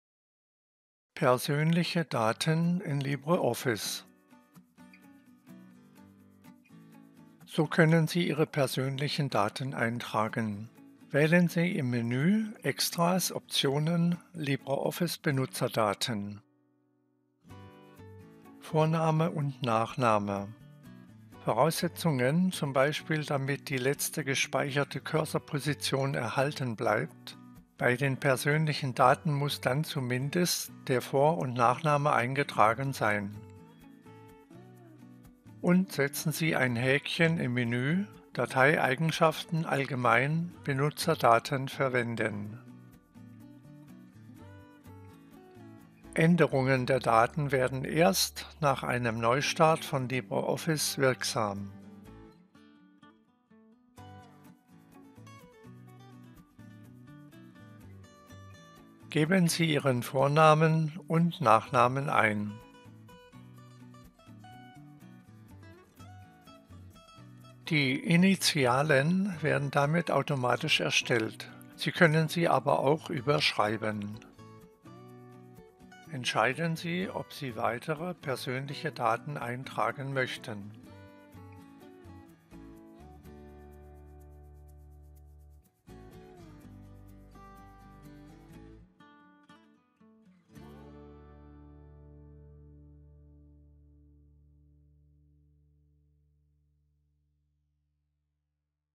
Persönliche Daten in LibreOffice 7.3 (German/Deutsch)
Dieses Video zeigt Ihnen, wie Sie Ihre persönlichen Daten in LibreOffice verwalten können.